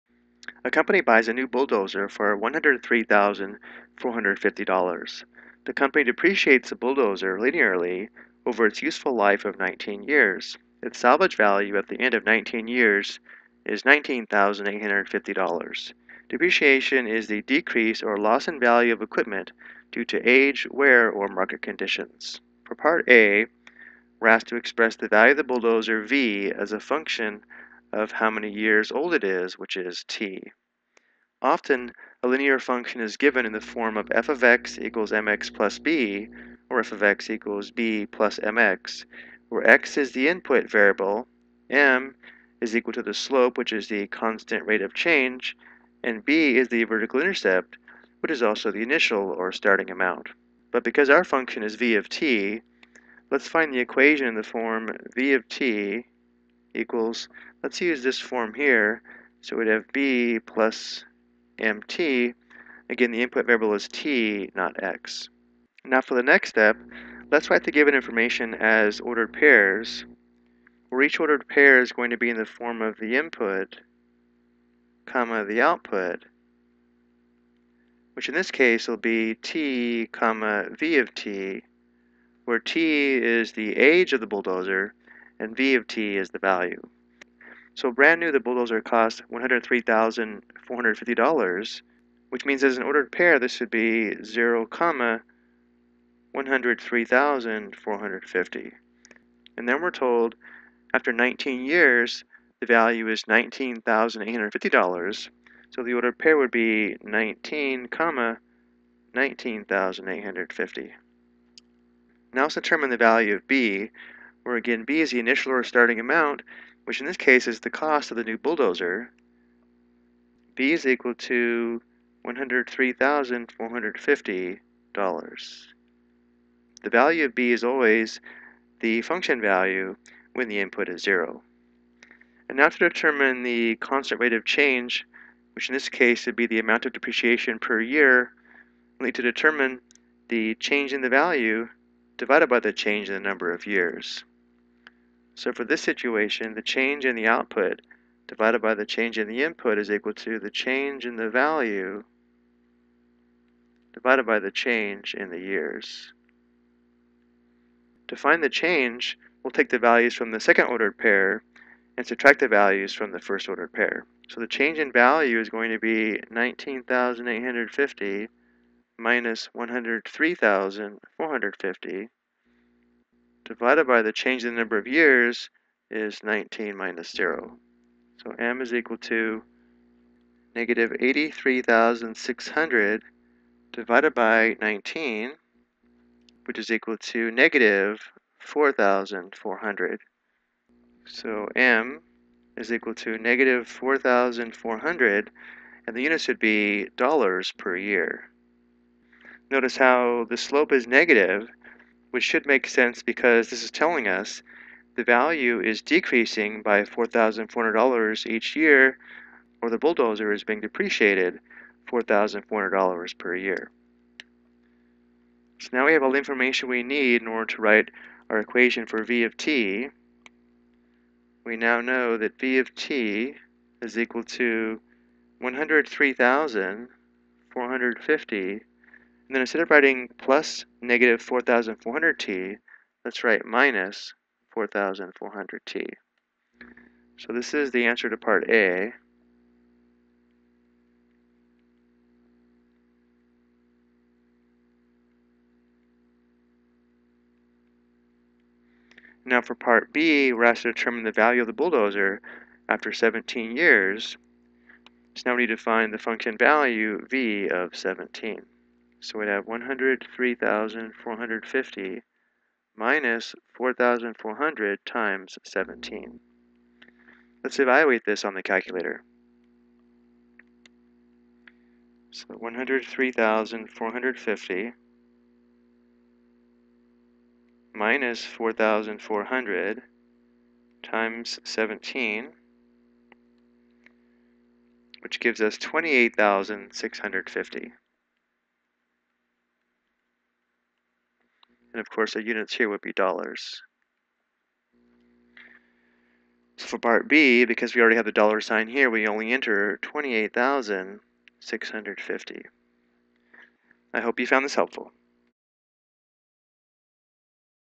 Ex: Model Depreciation Using a Linear Function (Determine Slope)
This video explains how to model depreciation using a linear function.  The function is then used to make predictions.  The slope must be calculated.